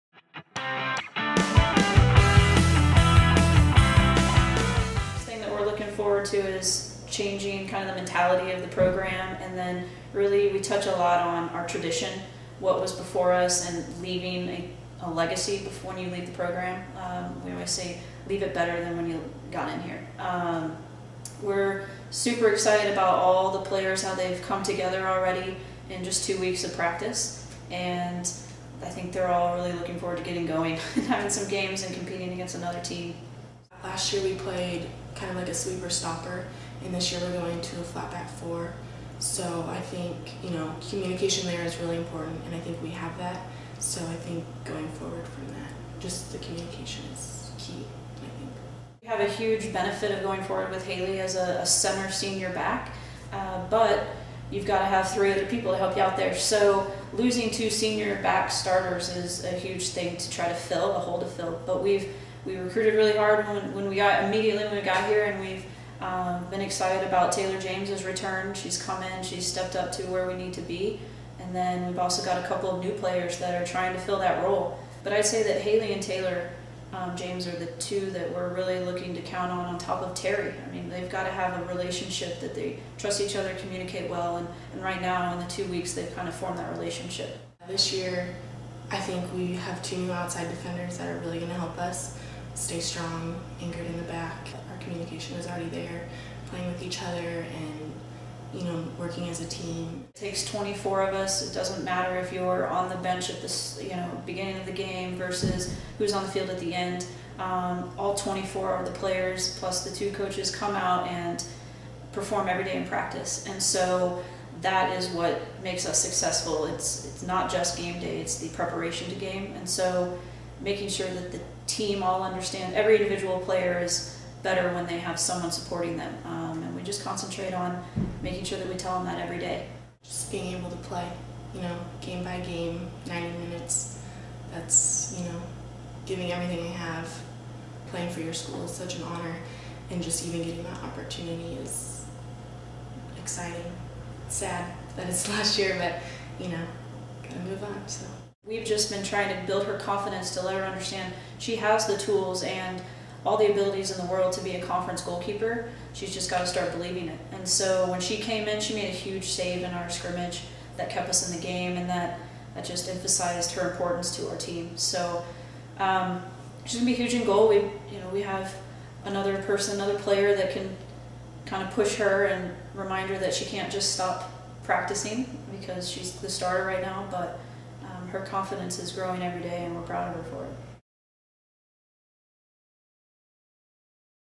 2014 Rogers State Women's Soccer Preview - Defenders/Goalkeepers
Head Coach Heather Cato and senior captain Haley Wenlock preview the 2014 season which gets underway Sept. 4 at Southern Nazarene.